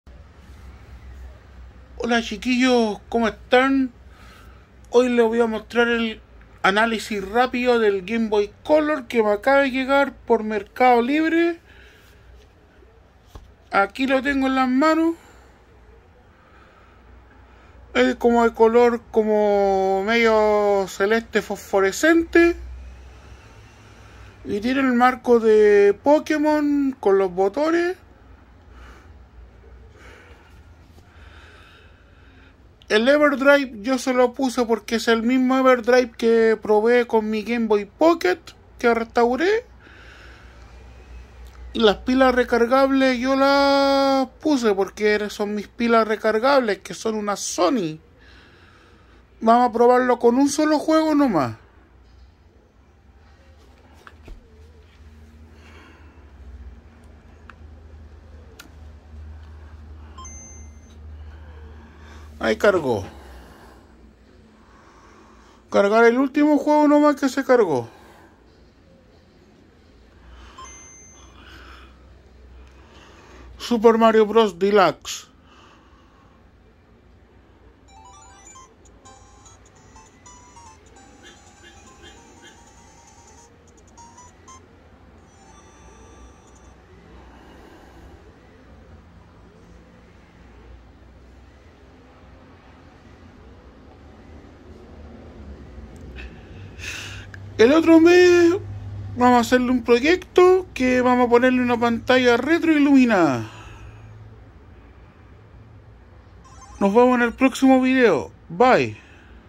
ANALISIS RAPIDO DE GAME BOY COLOR.
PROBANDO LA GAME BOY COLOR RECIEN COMPRADA EN MERCADO LIBRE.